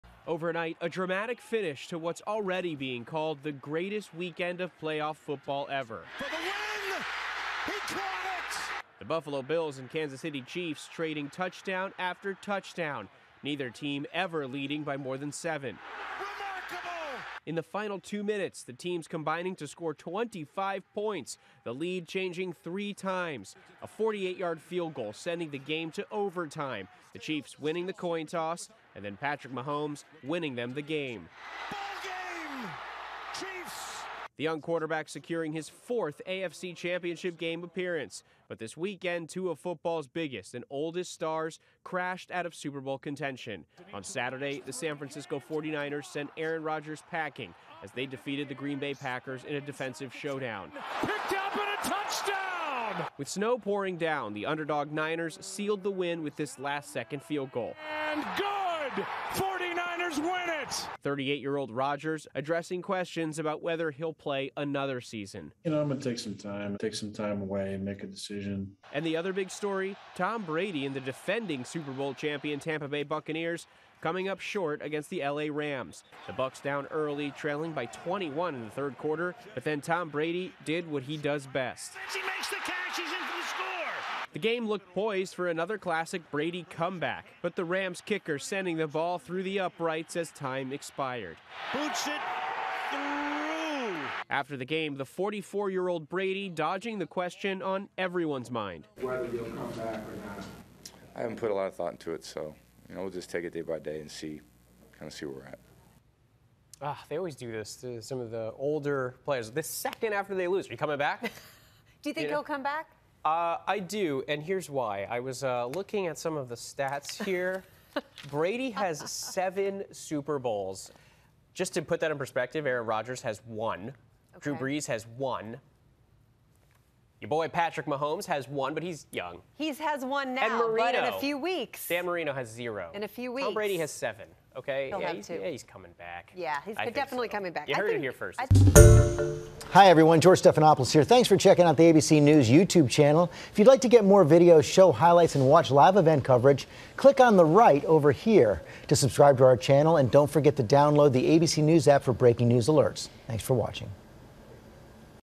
An epic NFL weekend
There are just four teams left, as Tom Brady and Aaron Rodgers contemplate their futures. ABC News’ Andrew Dymburt has more.